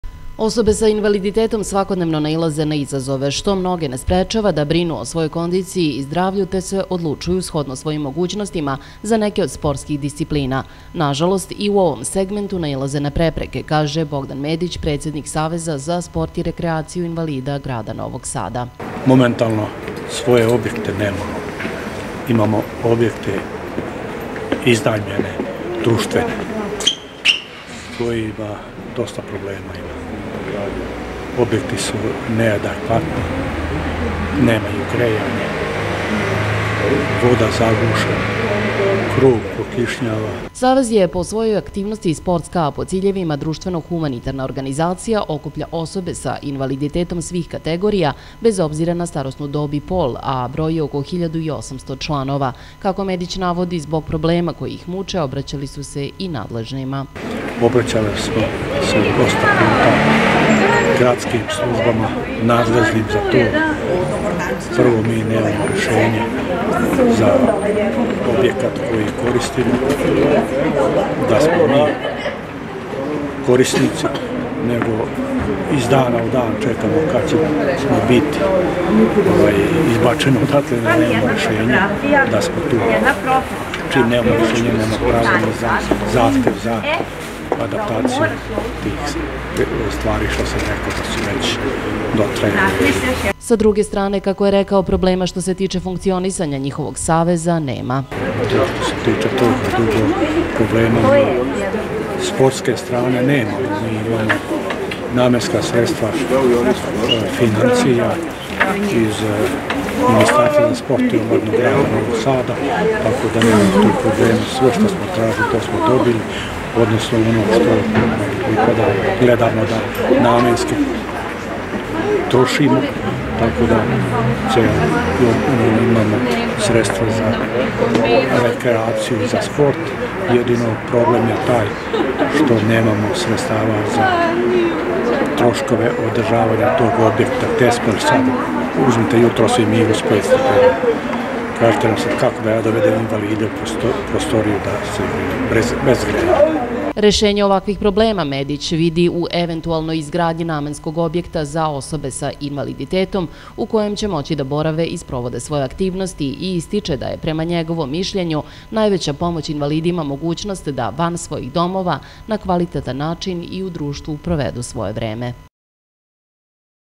TV KANAL 9, NOVI SAD: OSI POTREBNO MESTO ZA REKREACIJU
Najveći problem Saveza za sport i rekreaciju invalida Novog Sada je nedostatak adekvatnog prostora za boravak, mišljenje je predsednika ovog saveza Bogdana Medića.